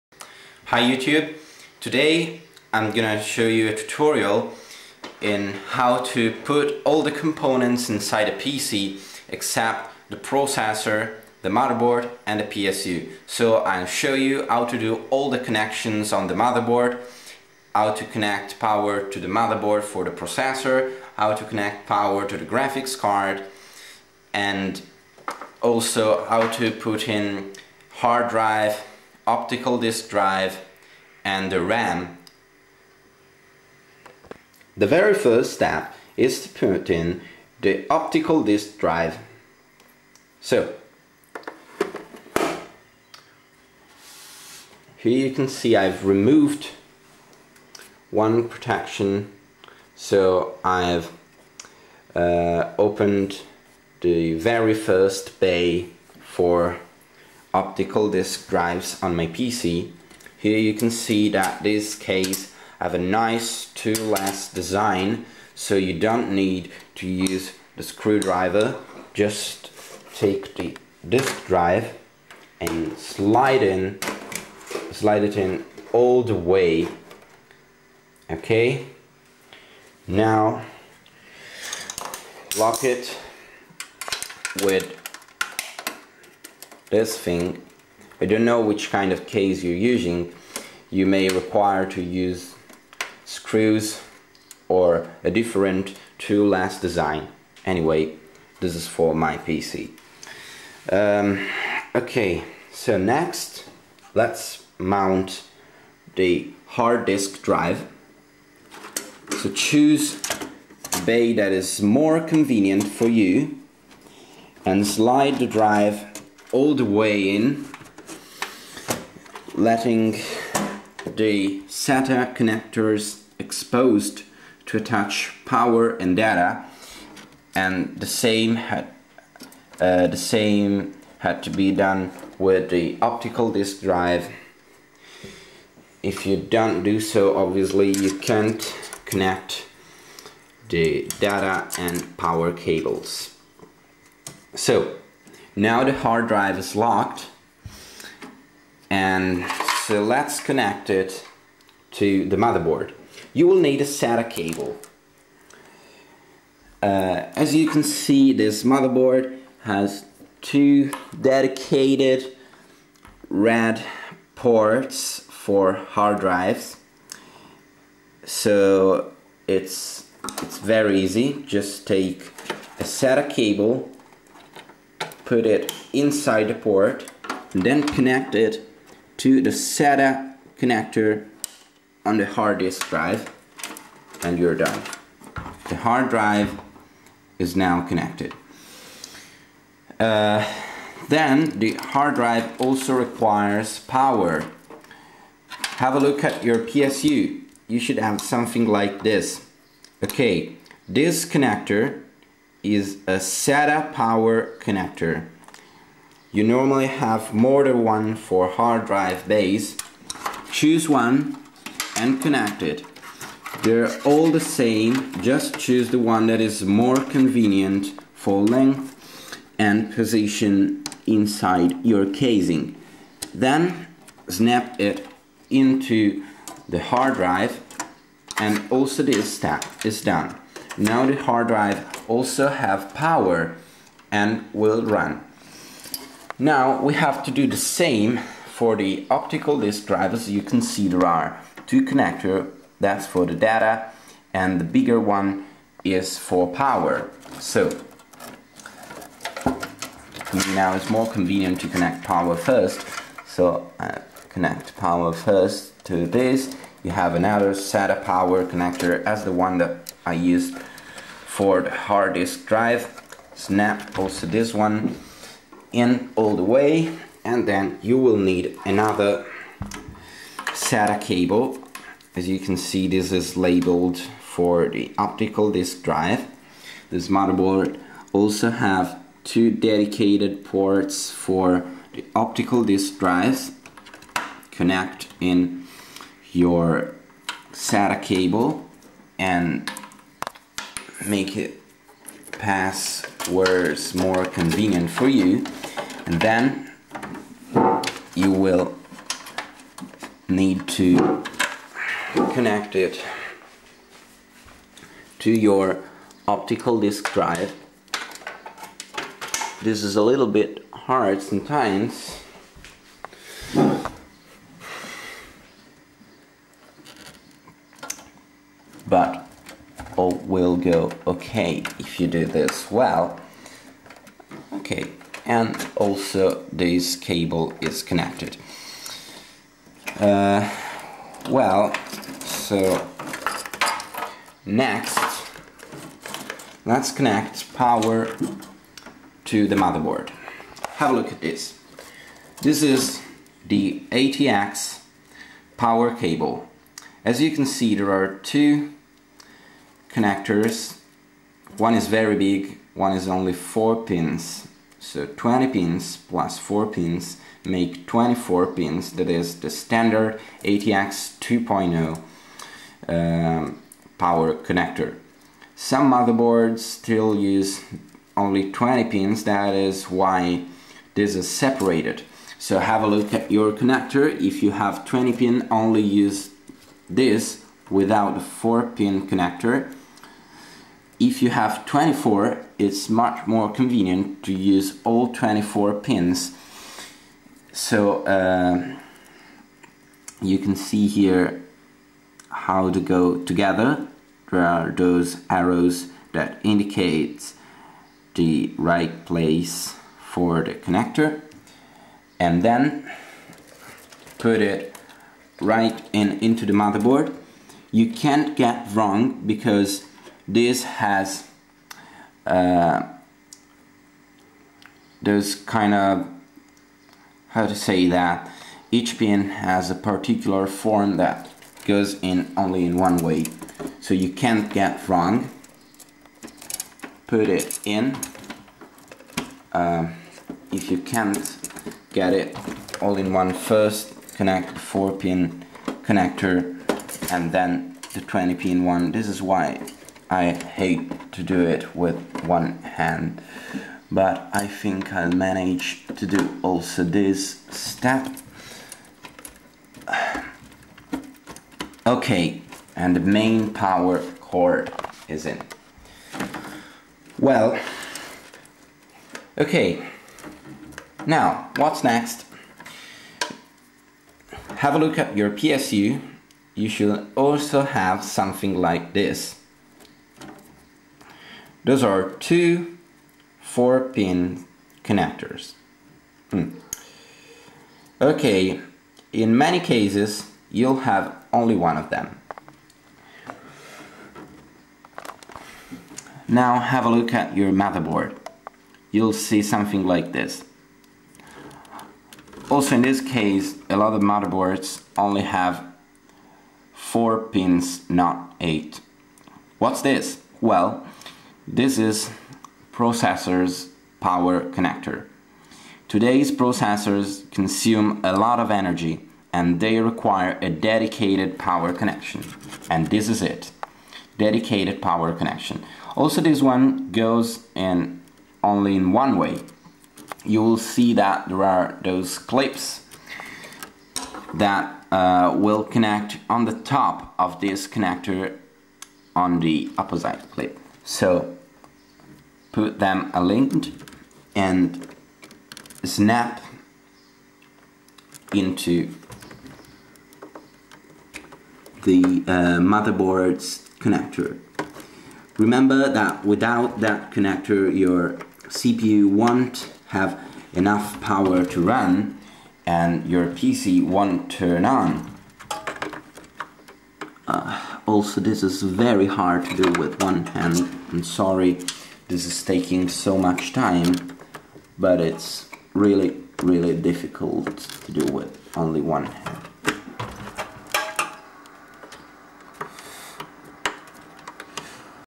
How to mount everything on your pc's motherboard (apart from the cpu) part1
In this tutorial I'll show you how to mount all the components on your pc's motherboard except the cpu, to mount the cpu refer to intel or amd website (depending on the processor you're going to use).
Last, but not least, if my English was bad, please correct me, I'm italian and I'm still learning =)